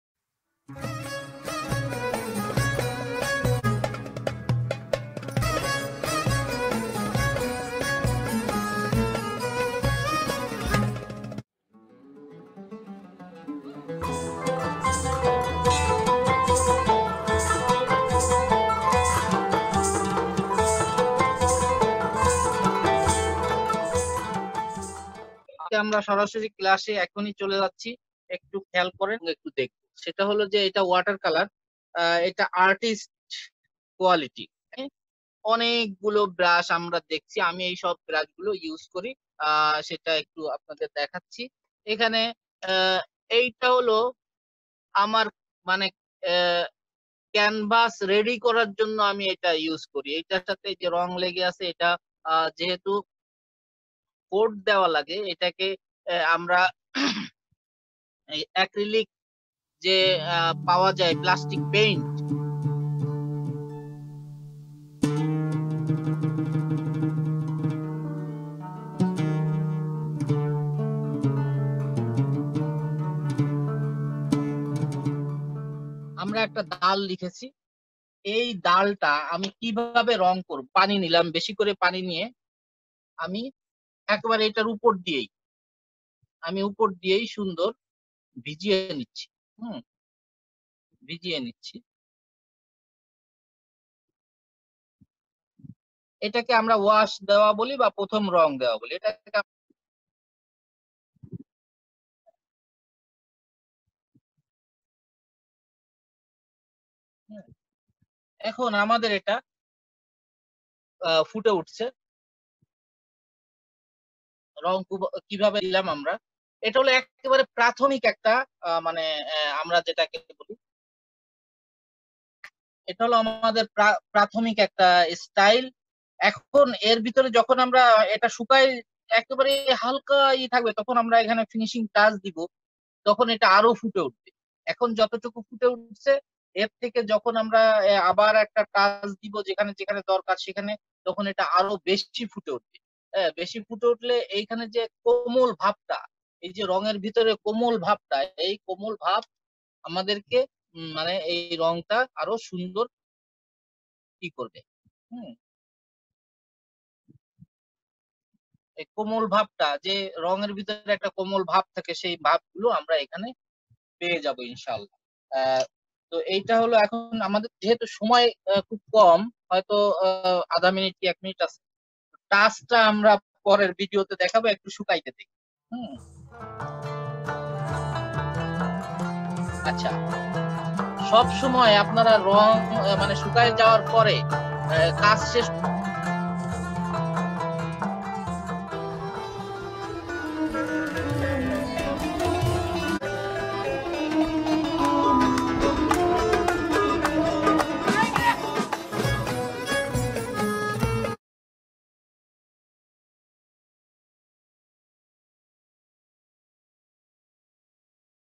Zoom OnlineCalligraphy Class 15 (Water color)
for all who love calligraphy art
arrenge by
Bangladesh Calligraphy Foundation

Teacher :
Ustad Mohammad Abdur Rahim
Famous Calligrapher of Bangladesh